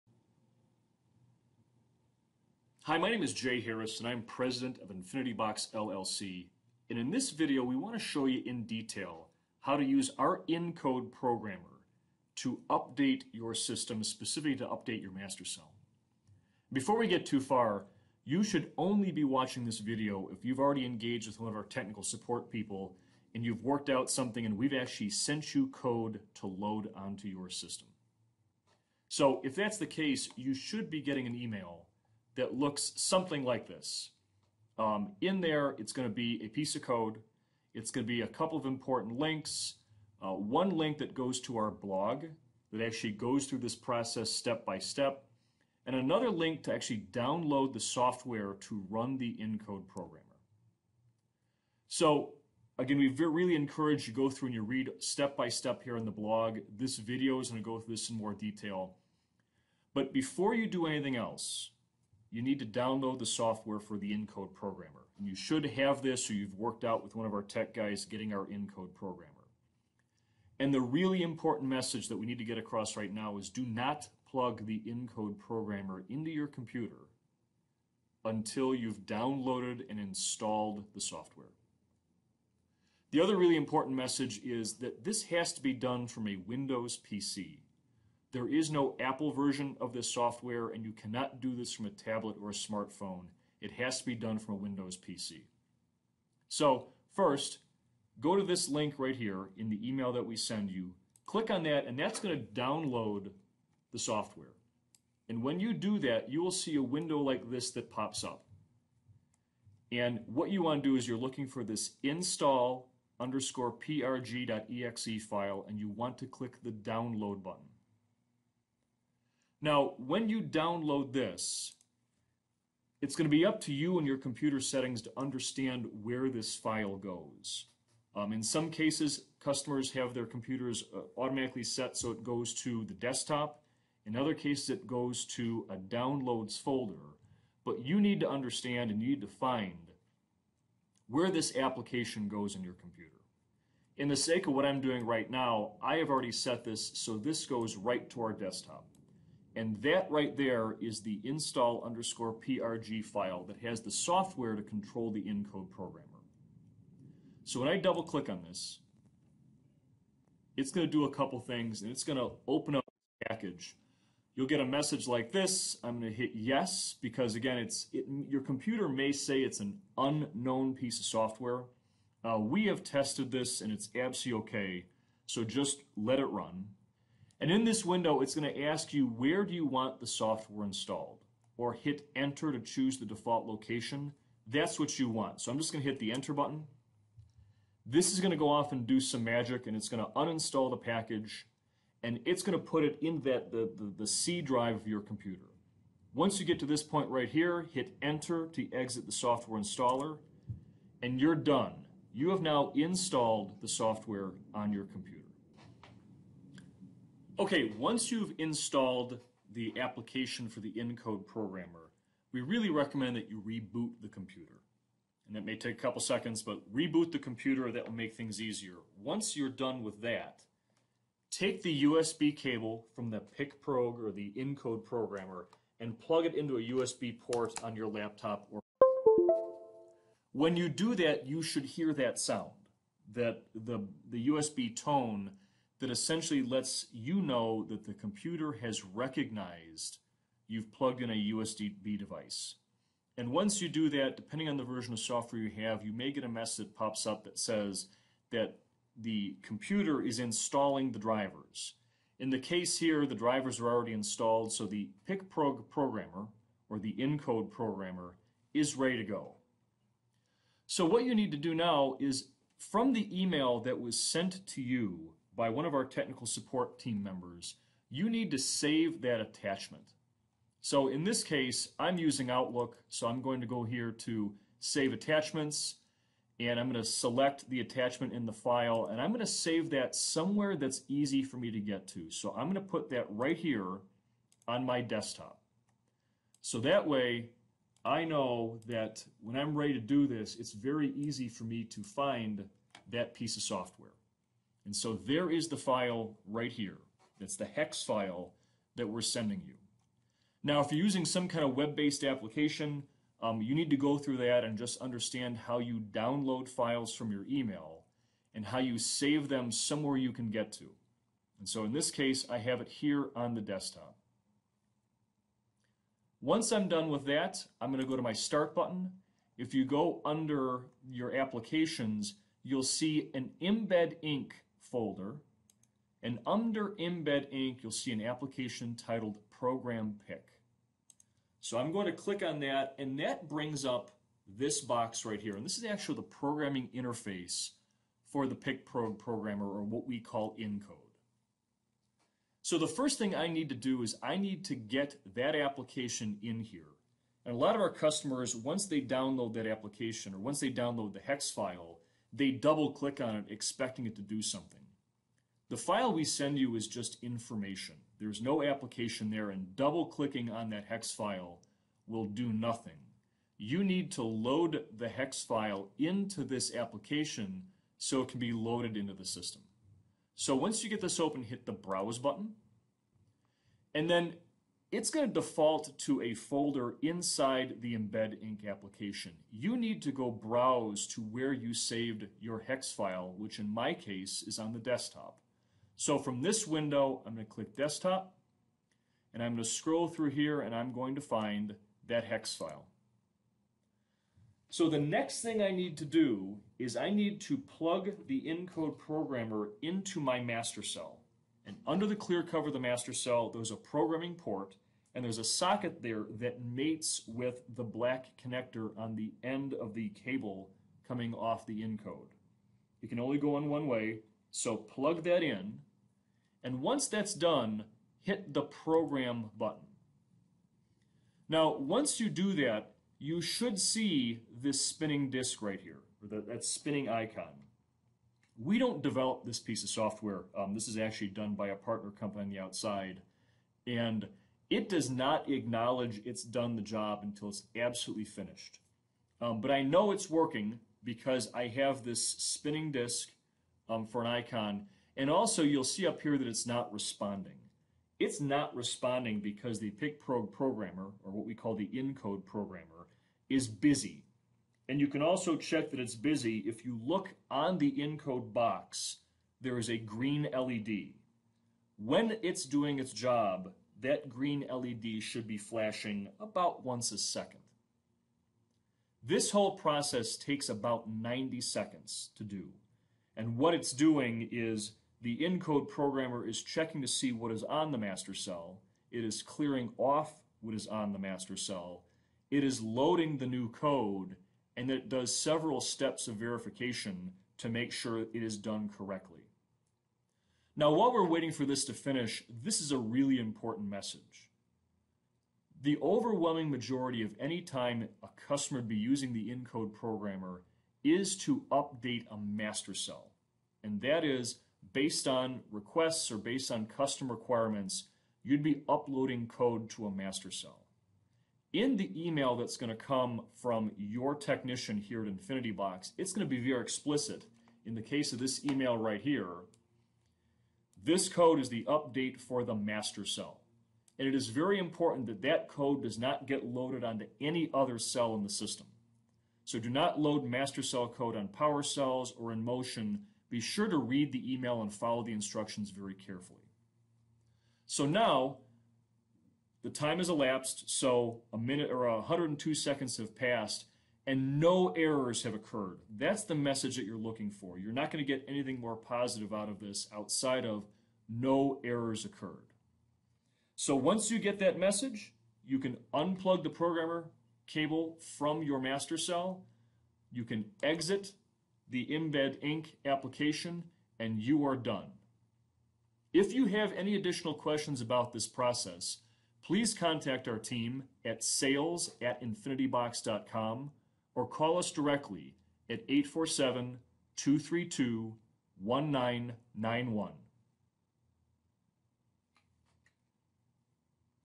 Infinitybox inCODE Programmer Demo Video
This video shows the steps to update an Infinitybox system using the inCODE programmer.  We show how to download the PIC-PROG application, install the application, download the HEX file and use inCODE to load it.